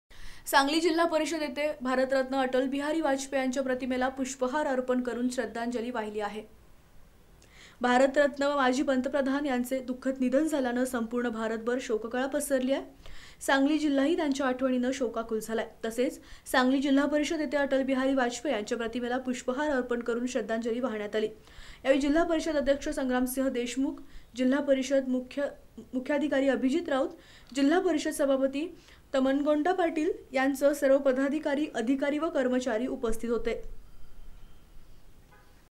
सांगली जिल्हापरिषद येथे भारतरत्न अटल बिहारी वाजपेयी यांच्या प्रतिमेला पुष्पहार
सांगली जिल्हापरिषद येथे भारतरत्न अटल बिहारी वाजपेयी यांच्या प्रतिमेला पुष्पहार अर्पण करून वाहिली श्रद्धांजली.@SBN मराठी.